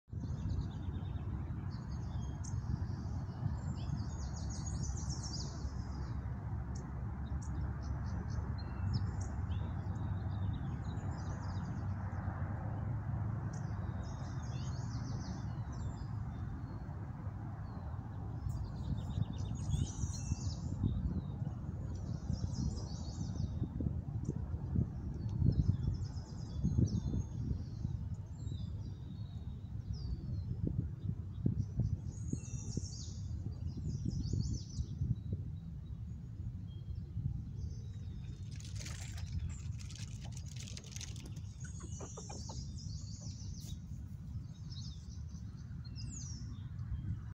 2 Squirrels didn't know each other existed until @0:35 Squirrel Chase 060221
All 3 squirrels gone!!